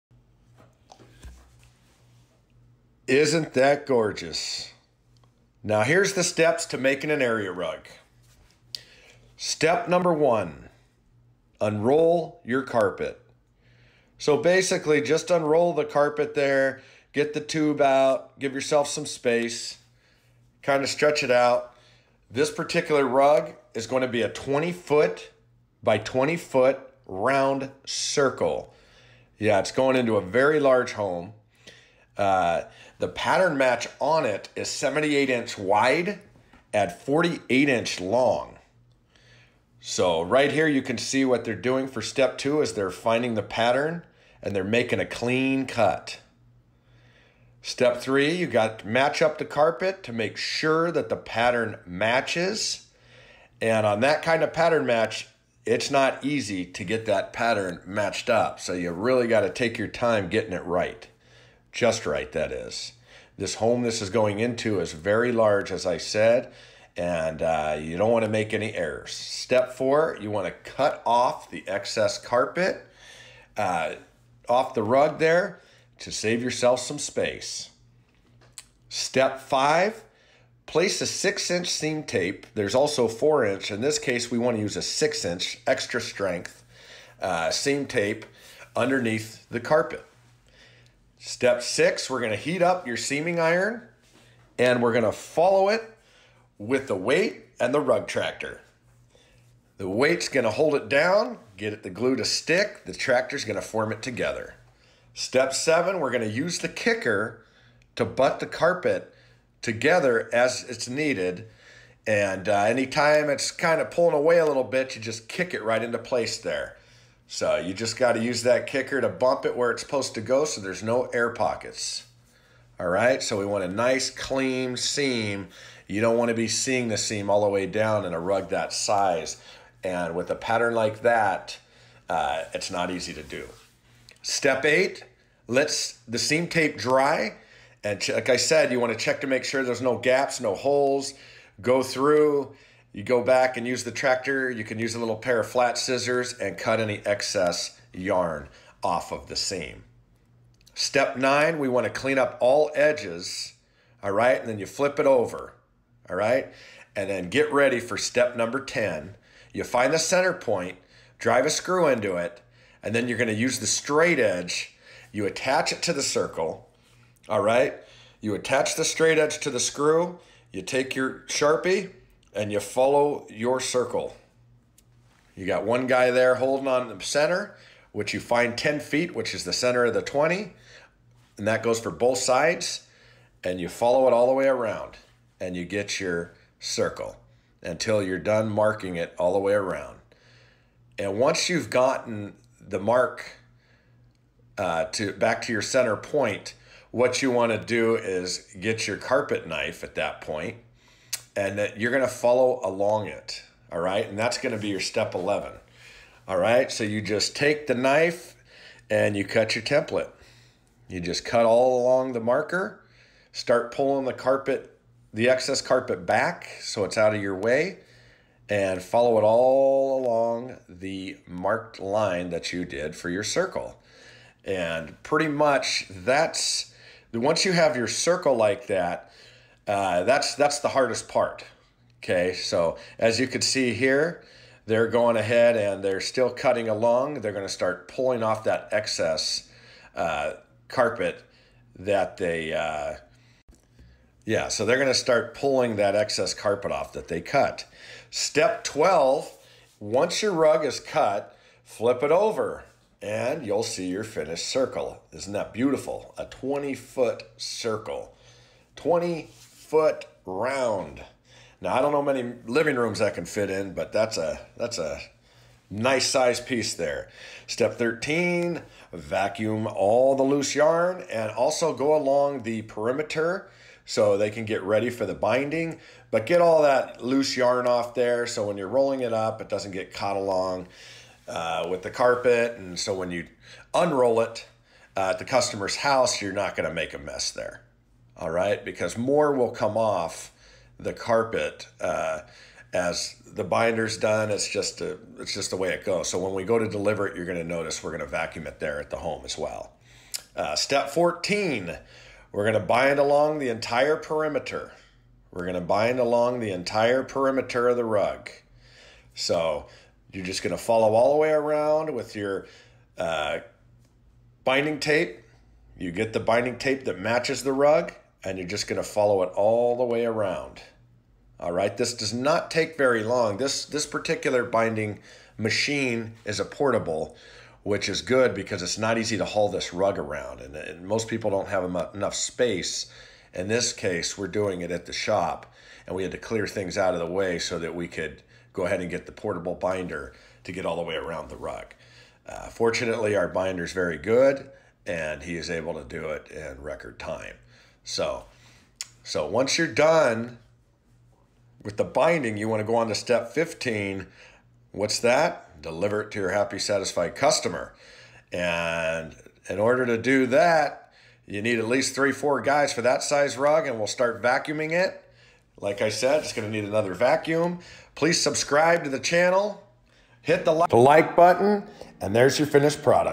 How to make a custom area rug with binding
Watch this step by step video and make your own beautiful rug!
This video educates people how to make an area rug by cutting, seeming, and binding.